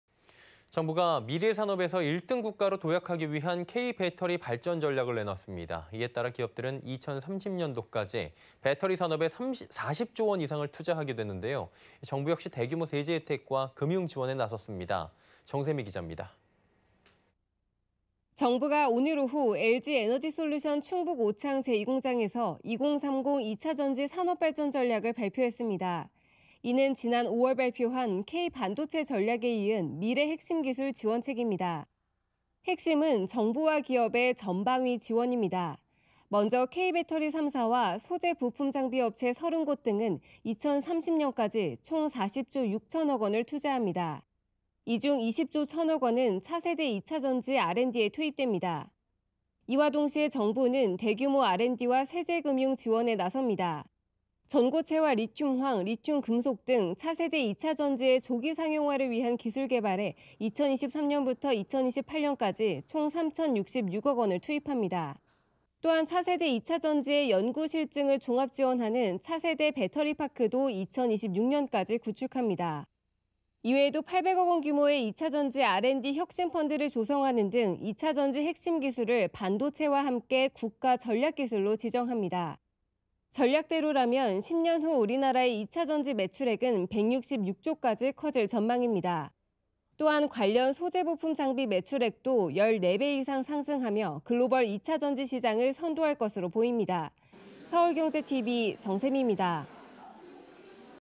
정부·기업 ‘K-배터리’ 종합 지원…40조 투자
[앵커]
정부가 미래 산업에서 1등 국가로 도약하기 위한 'K-배터리 발전 전략'을 내놨습니다. 이에 따라 기업들은 2030년까지 배터리 산업에 40조원 이상을 투자하는데요. 정부 역시 대규모 세제 혜택과 금융 지원에 나섭니다. 보도에 정새미 기자입니다.

[서울경제TV=정새미 기자]

정부가 오늘 오후 LG에너지솔루션 충북 오창 제2공장에서 '2030 이차전지 산업(K-배터리) 발전 전략'을 발표했습니다.

이는 지난 5월 발표한 ‘K-반도체 전략’에 이은 미래 핵심 기술 지원책입니다.

핵심은 정부와 기업의 ‘전방위 지원’입니다.

먼저 K배터리 3사와 소재·부품·장비 업체 30곳 등은 2030년까지 총 40조6,000억원을 투자합니다.

이 중 20조1,000억원은 차세대 이차전지 R＆D에 투입됩니다.

이와 동시에 정부는 대규모 R＆D와 세제·금융 지원에 나섭니다.

전고체·리튬황·리튬금속 등 차세대 이차전지의 조기 상용화를 위한 기술개발에 2023년부터 2028년까지 총 3,066억원을 투입합니다.

또한 차세대 이차전지의 연구·실증을 종합 지원하는 ‘차세대 배터리 파크’도 2026년까지 구축합니다.

이 외에도 800억 원 규모의 이차전지 R＆D 혁신펀드를 조성하는 등 이차전지 핵심기술을 반도체와 함께 국가전략기술로 지정합니다.

전략대로라면 10년 후 우리나라의 이차전지 매출액은 166조까지 커질 전망입니다.

또한 관련 소재·부품·장비(소부장) 매출액도 14배 이상 상승하며 글로벌 이차전지 시장을 선도할 것으로 보입니다.

[영상편집 김준호]

[SENTV]